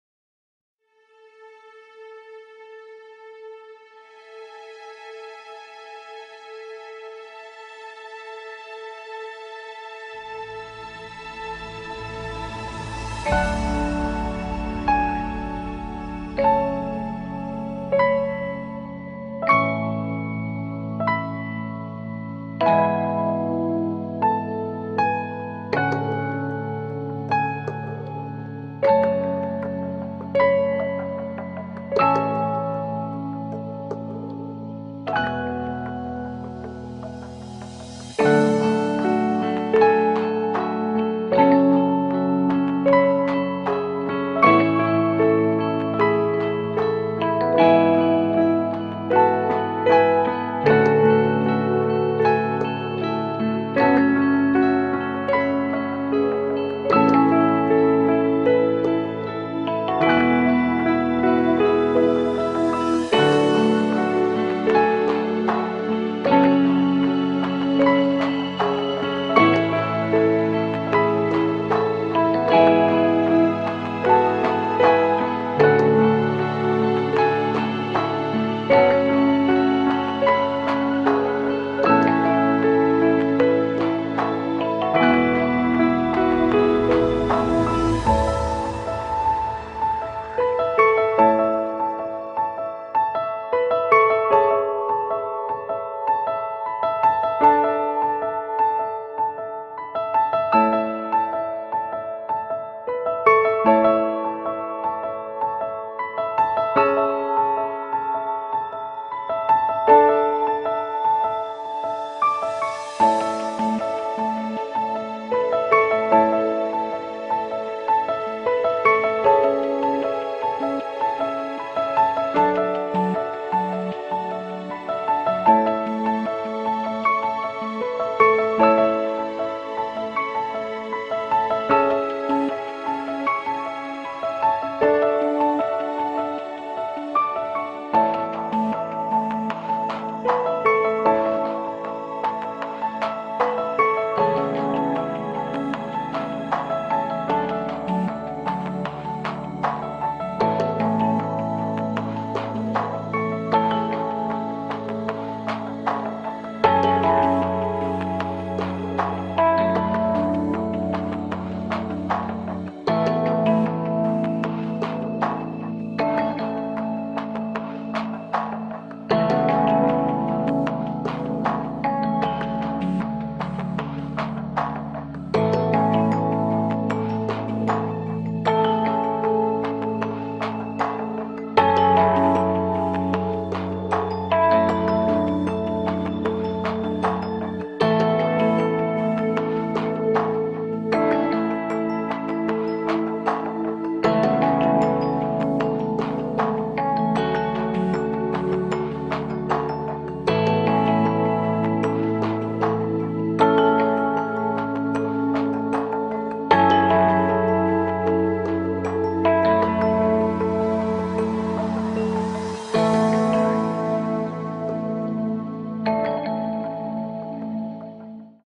Concentration Music - Work and Study better
This kind of music helps you concentrate and focus better and it will help you work more efficient.
- Baroque music / Mozart music
Relaxing Music.